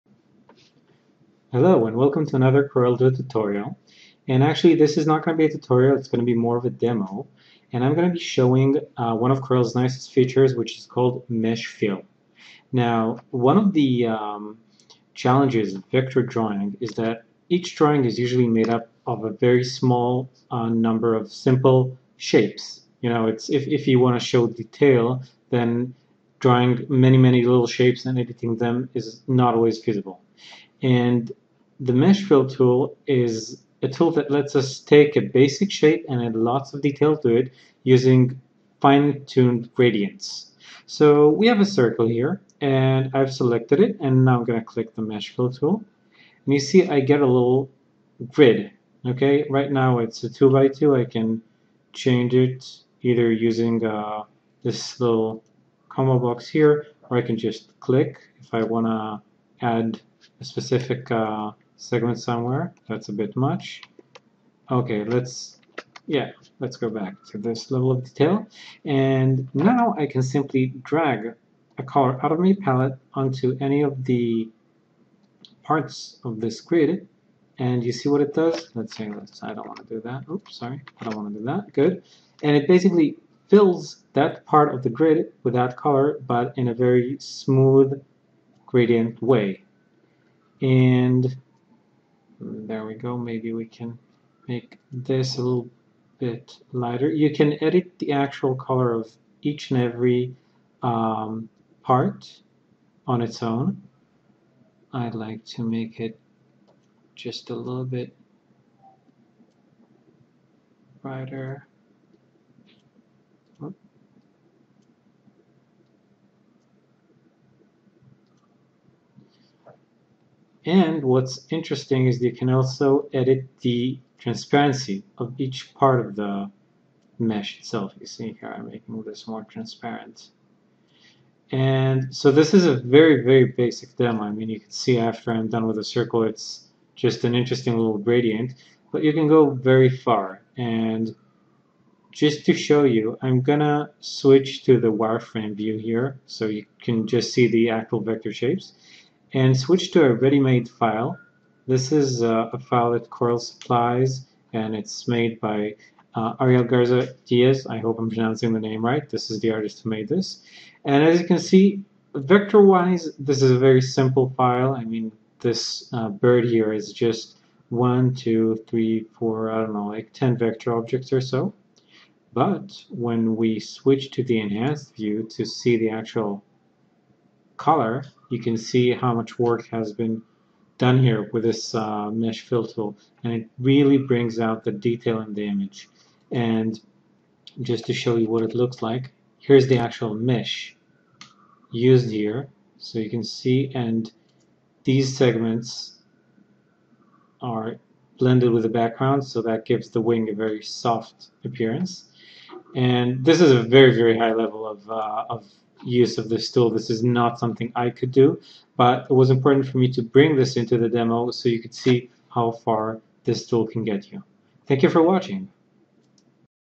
CorelDraw X5 Demo: What the Mesh Fill tool can do
This is a simple demo showing the Mesh Fill tool in CorelDraw X5. It is aimed at people who have never used a vector drawing application before, just to show what this tool can do.